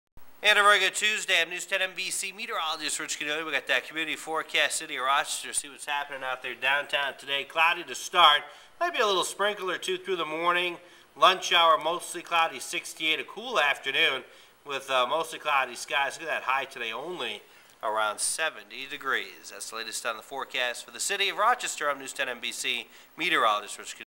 City forecast for September 3rd
News 10 NBC Meteorologist Rich Caniglia pinpoints today's City of Rochester community weather forecast.